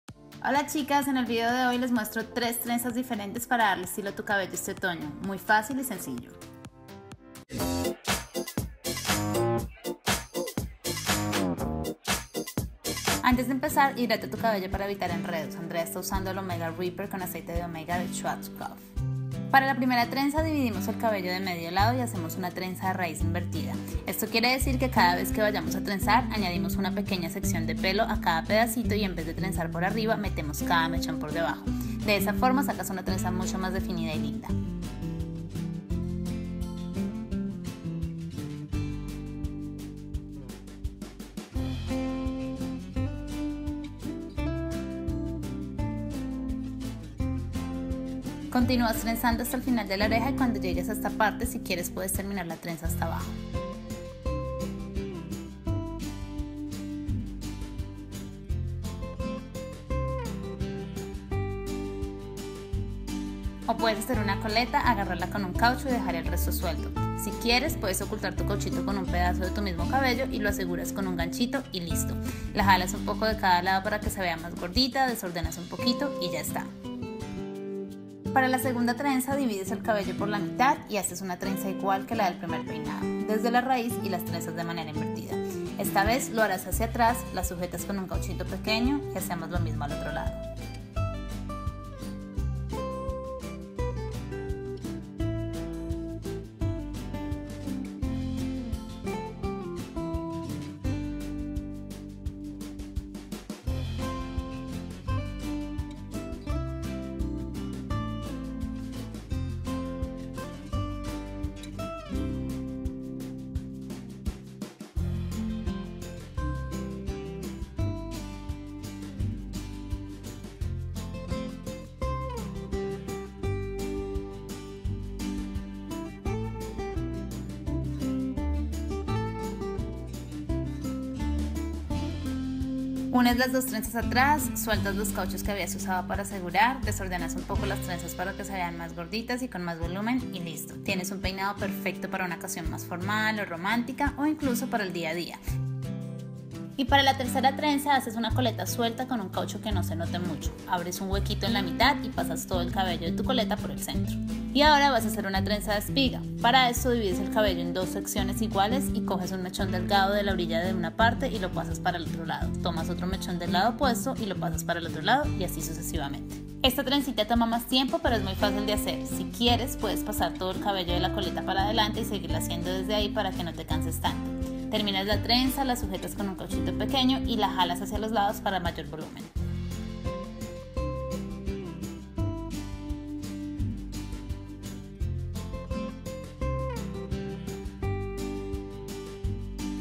3 Trenzas fáciles | Otoño - KeepItCaro
¿Quieres darle estilo a tu cabello este otoño?

A todas nos encanta arreglar nuestro cabello, pero muchas veces no tenemos el tiempo suficiente para peinarlo como quisiéramos todos los días. Por eso, en mi video de hoy, te muestro 3 trenzas diferentes y muy fáciles de hacer, perfectas para llevar en otoño.

Así que tienes que ver el video y probarlas todas!!!

Si te gusta el vídeo déjame tu like y comenta cuál es tu favorita!

Si aún no estás suscrita, únete a esta fabulosa comunidad!

Caro

SÍGUEME!

CONSIGUE EL OMEGA REPAIR!